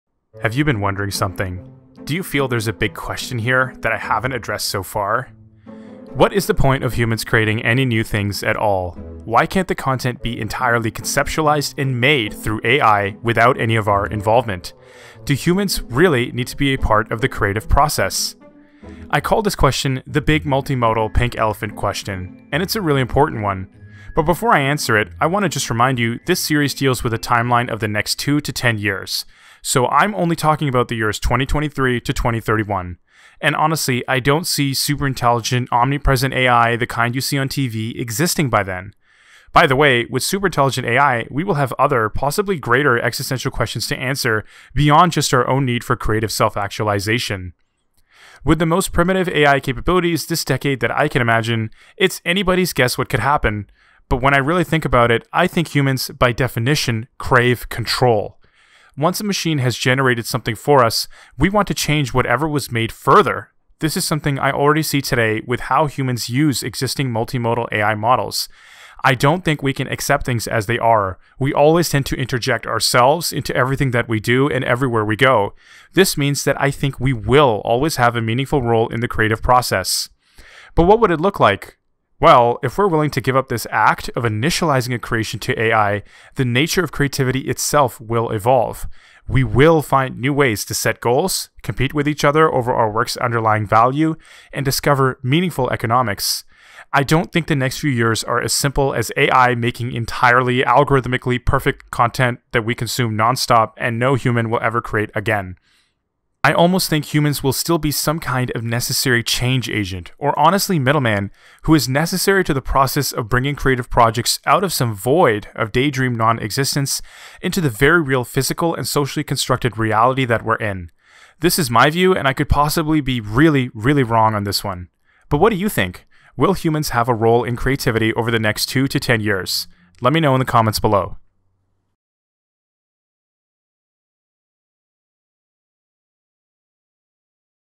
#7 - The Big, Unanswered Question about Multimodal Creativity
Have you been wondering something so far in this series which I haven't addressed?  In this video, I talk my view on the future role of human creativity.

This video is a part of a YouTube Playlist called, "GPT-X, DALL-E, and our Multimodal Future".